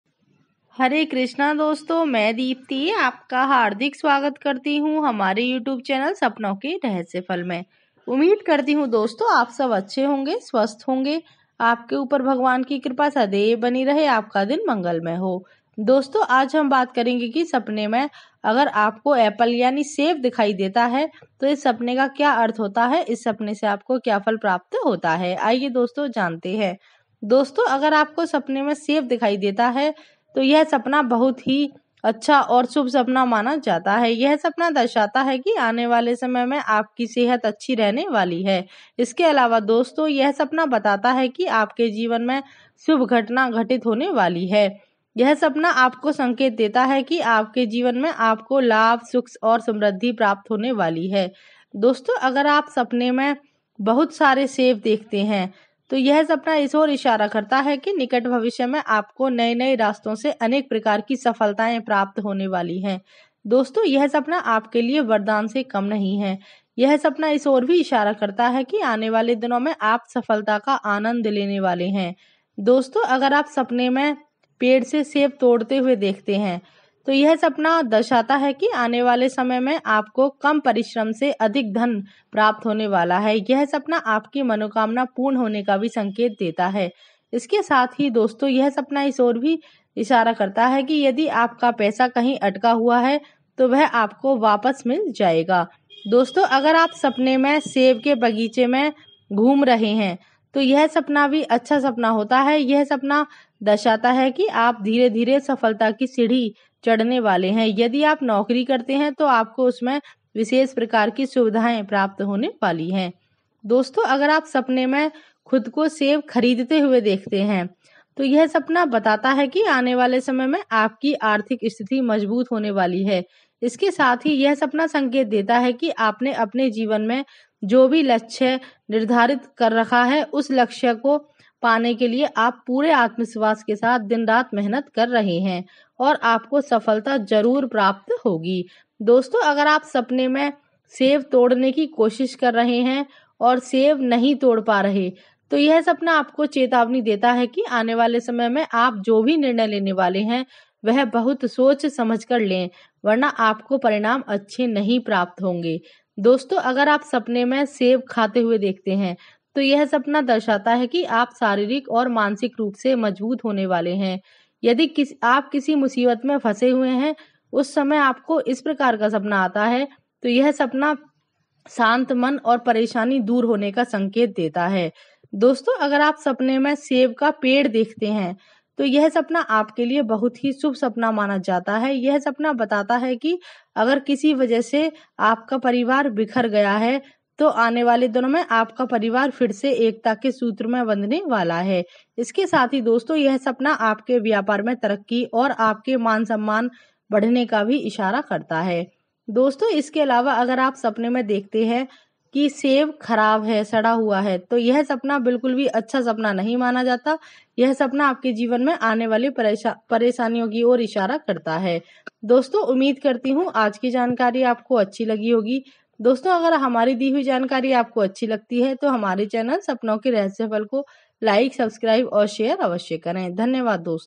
सपने में सेव देखने का अर्थ#seeing Apple in Dreams#sapnokarahasyafal ##
हरे कृष्ण दोस्तो🙏🙏
सपने में सेव देखने का अर्थ और उसका फल.
दोस्तो आपकी support की हमे बहुत जरूरत है🙏🙏🙏